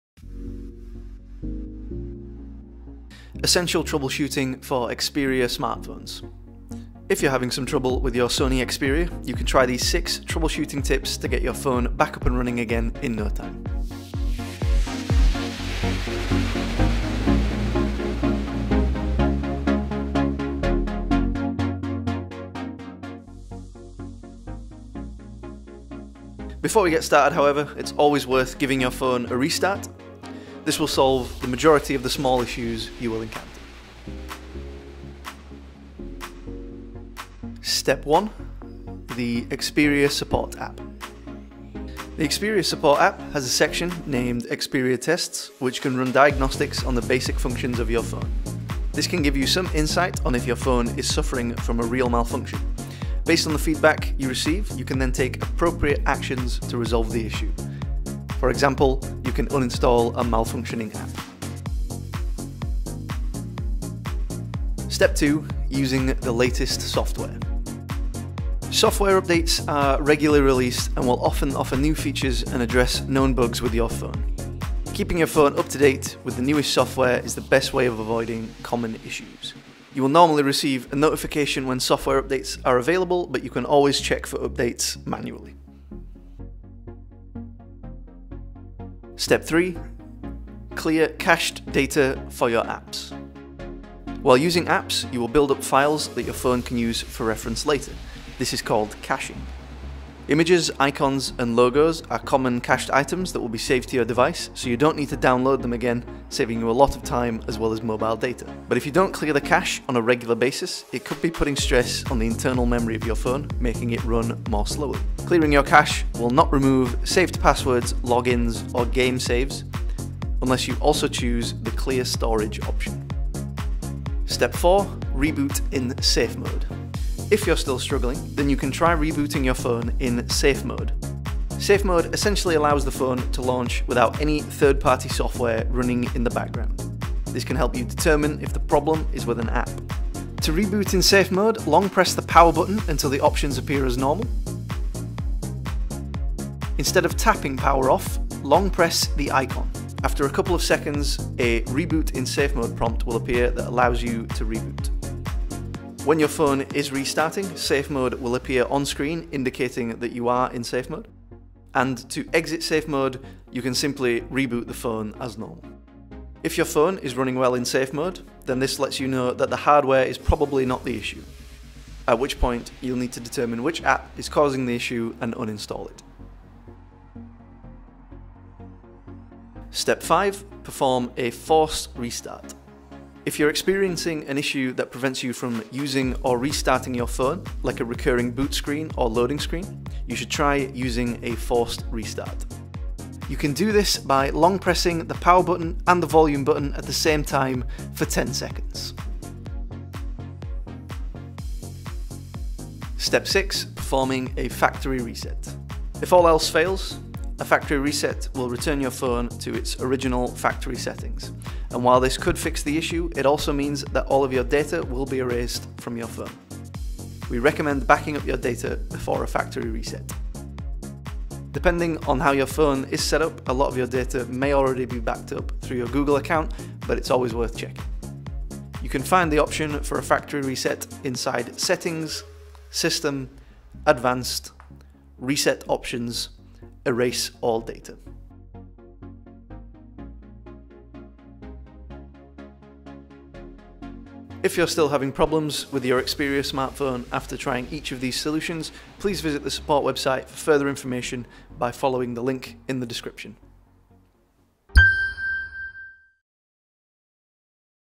How to troubleshoot your Sony Xperia smartphone
Having trouble with your Sony Xperia? Try these six troubleshooting tips to get your phone up and running again. In this video tutorial, we cover:

00:36 - Xperia Support app
01:04 - Software update
01:28 - Clearing your cache
02:03 - Reboot in Safe Mode
02:58 - Force restart
03:22 - Factory reset